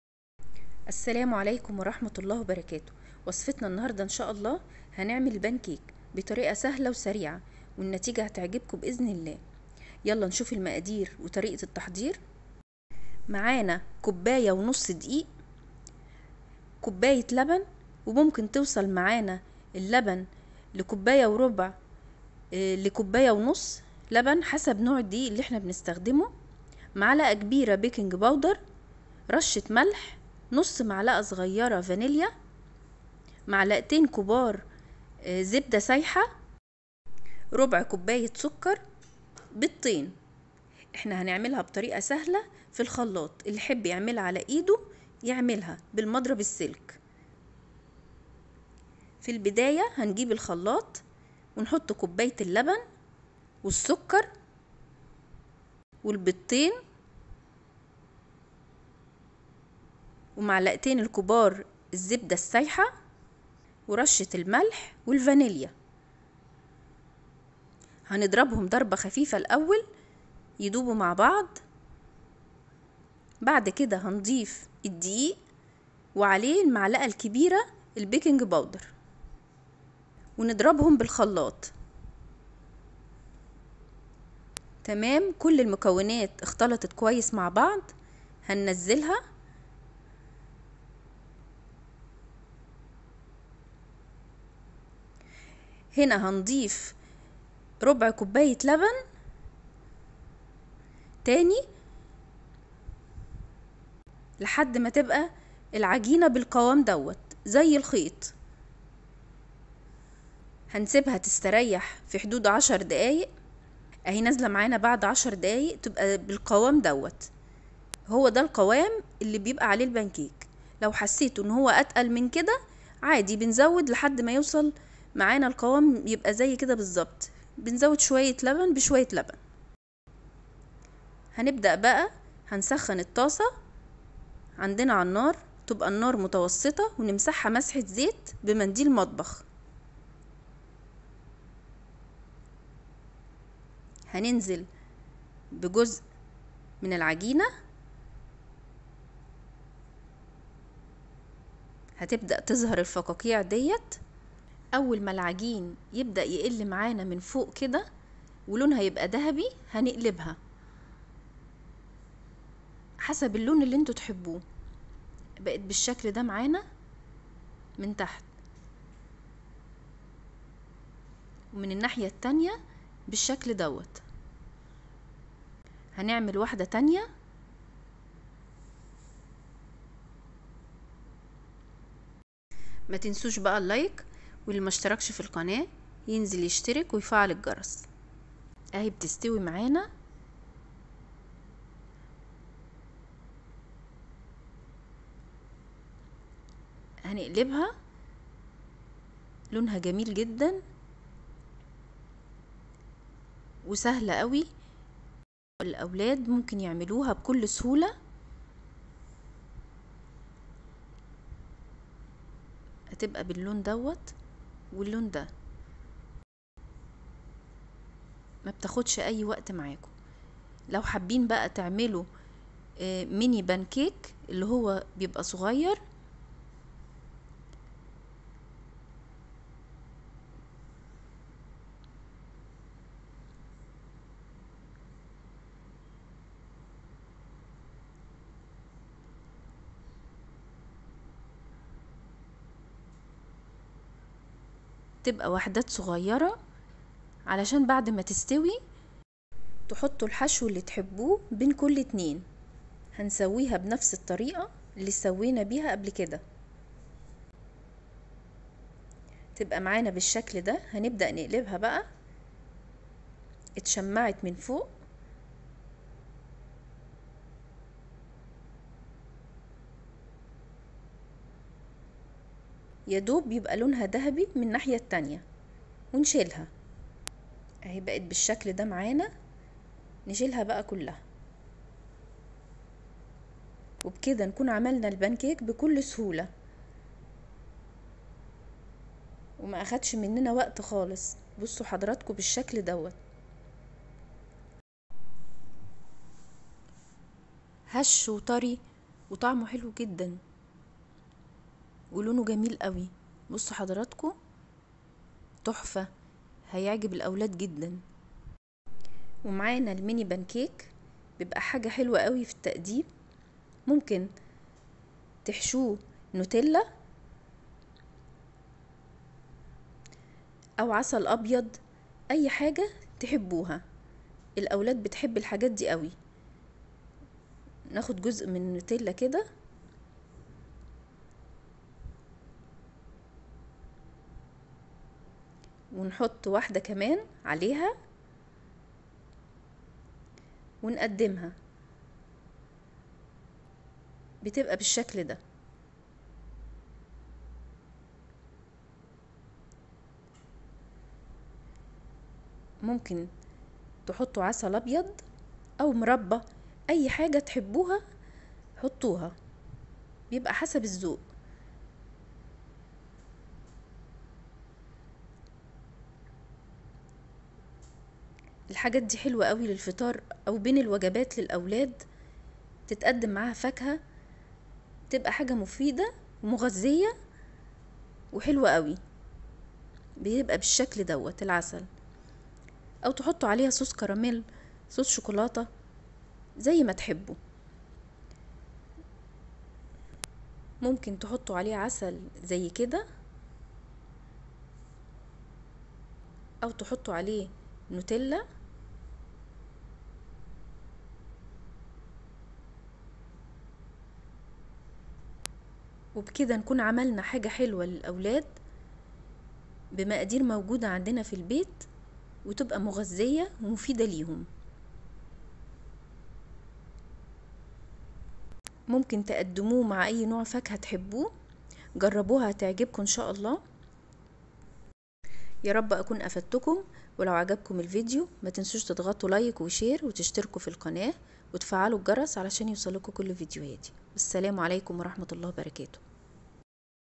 البان كيك الهش واللذيذ بطريقة سهلة وطعم روووعة 😋مروه مصطفى
1 (1/2) كوب دقيق
كوب الى كوب وربع لبن
1/4 كوب سكر
2 ملعقة كبيرة زبده سايحة
ملعقة كبيرة بيكنج بودر
1/2 ملعقة صغيرة فانليا
رشة ملح